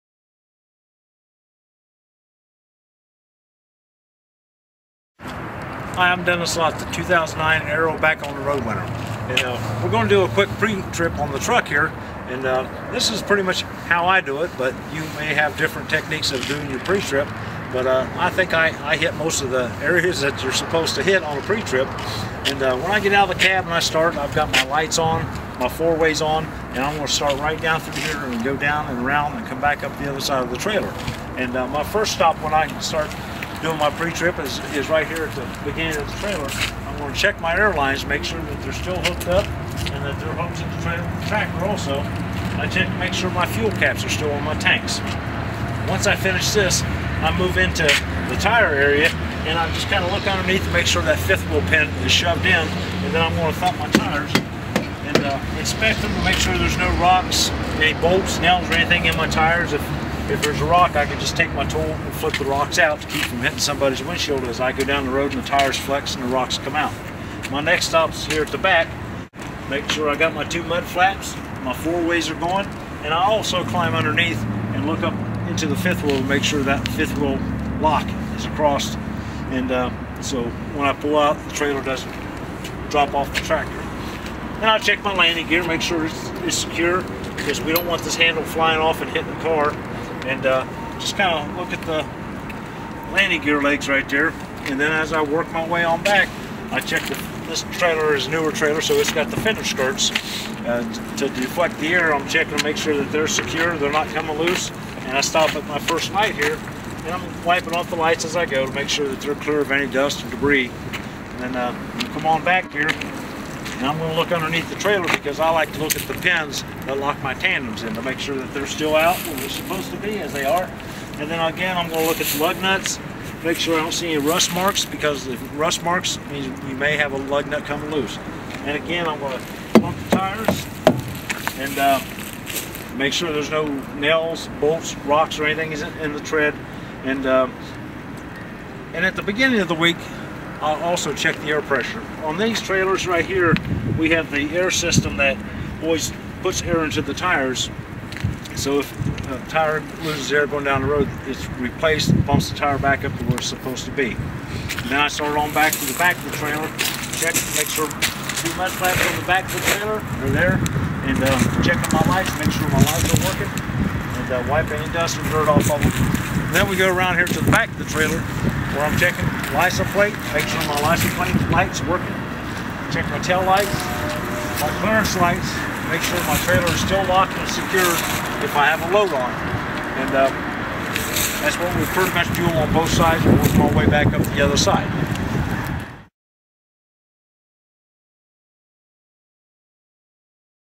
Pre Trip Inspection- Dennis Lott
Here's a short informational video where I discuss how to do a basic pre-trip inspection of your truck before starting your route. Thanks to Arrow Truck Sales for supporting my video blog!